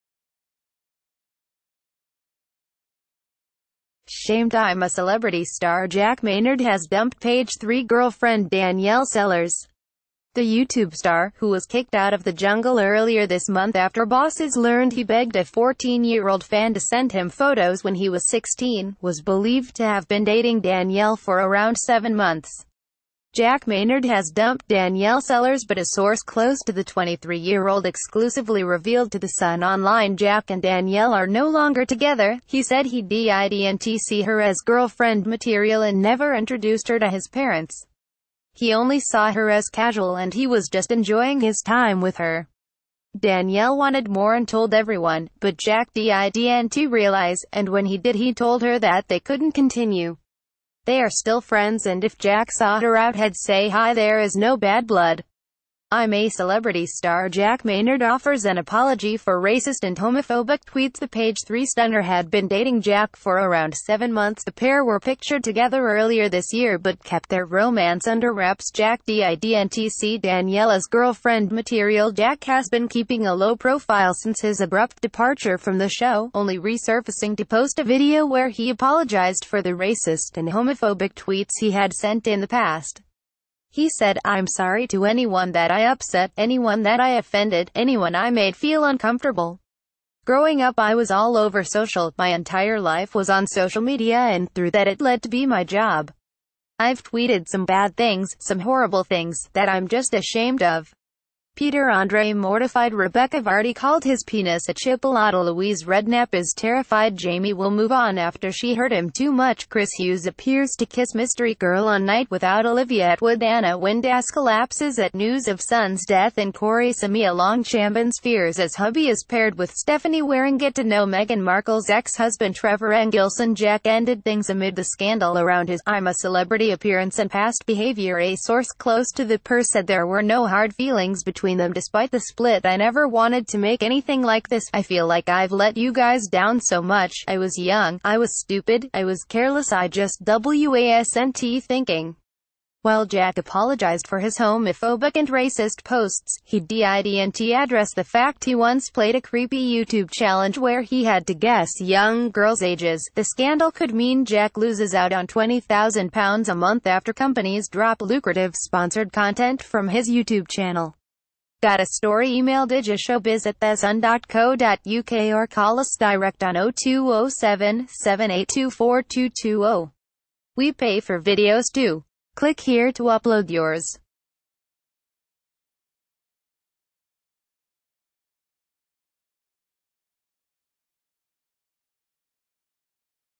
Shamed jack maynard dumps page 3 girlfriend danielle sellers after i'm a celeb scandal| Daily News
Shamed jack maynard dumps page 3 girlfriend danielle sellers after i'm a celeb scandal| Daily News TV
SHAMED Im a Celebrity star Jack Maynard has dumped Page 3 girlfriend Danielle Sellers. The YouTube star, who was kicked out of the jungle earlier this...